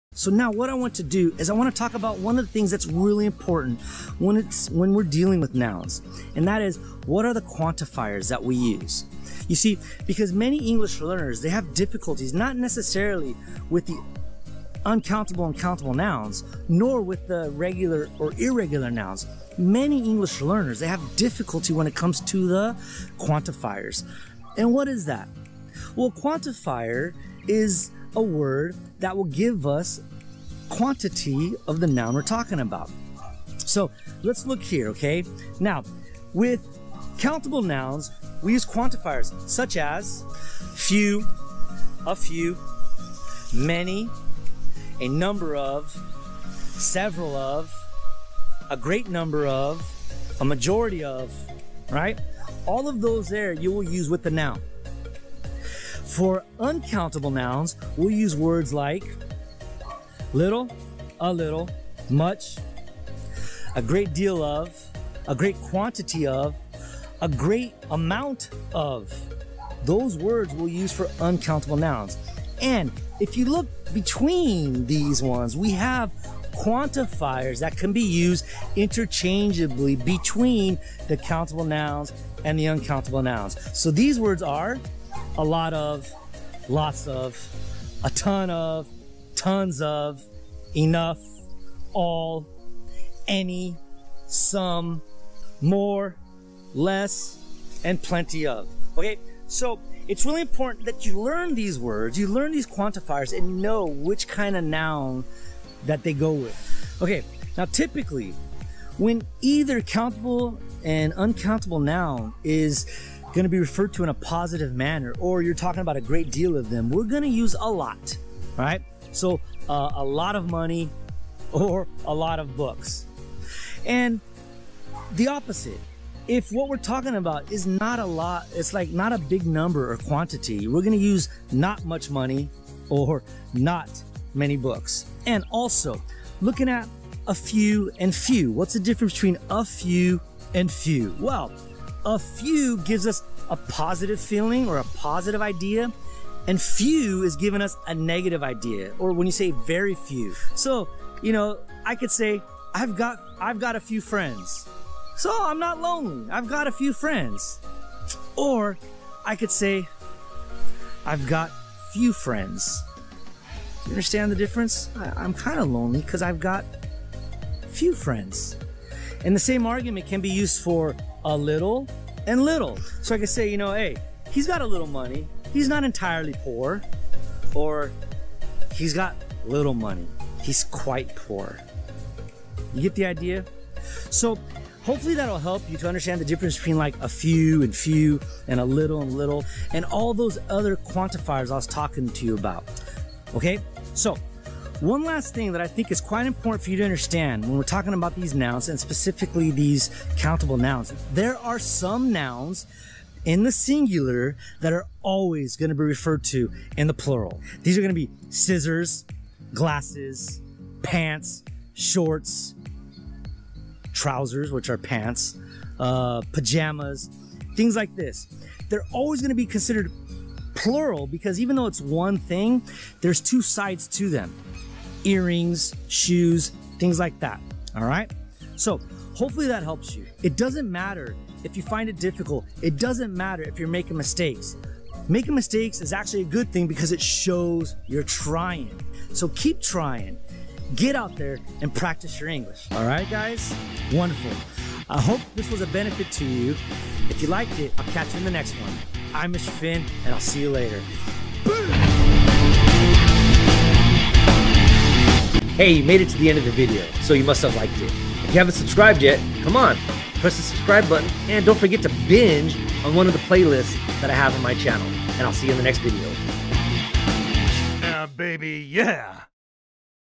Quantifiers "Many / Much / Few / Little / etc." | What are they and how to use them!
When it comes to learning about nouns and how to use them, most English learners struggle with QUANTIFIERS. Why? well, the simple answer is that there are so many. Another is they get confused between Which ones work for countable nouns, uncountable nouns, or both.

In this video I cover the variety of these quantifiers and their uses.